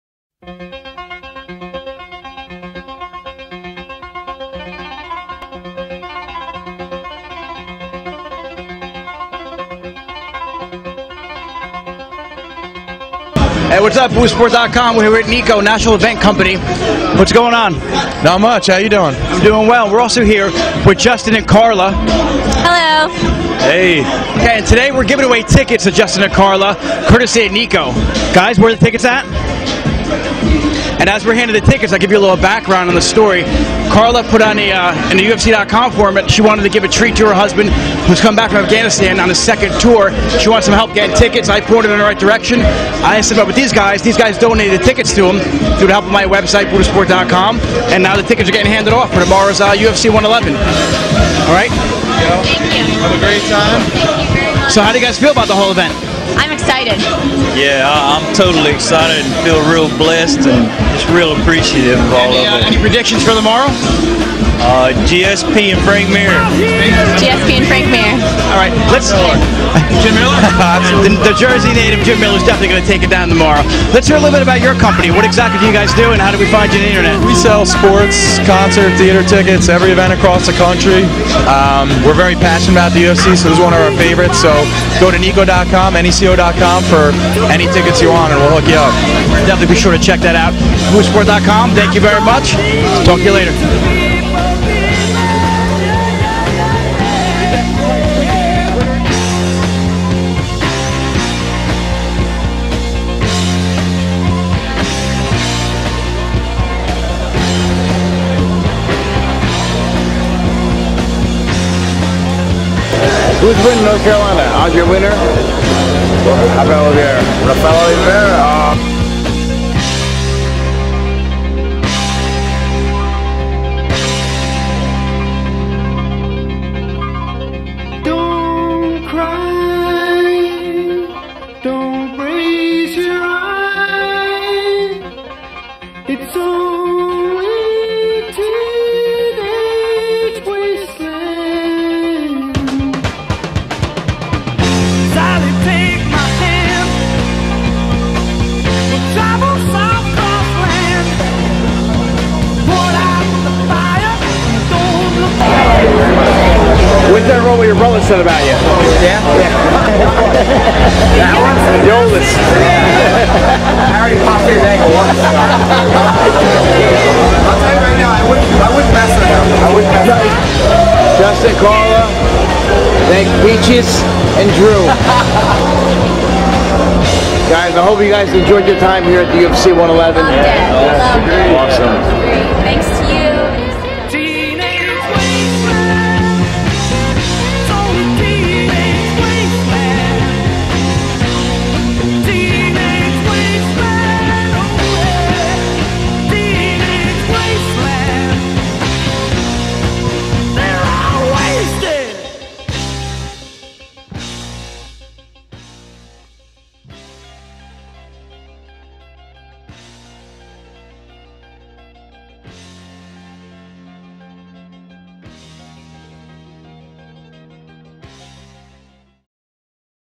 UFC 111 - A weekend for a Hero
- Buddhasport.com is a firm believer in helping all those who need a hand. Check out how we thanked one of brave soldiers for fighting for our freedom..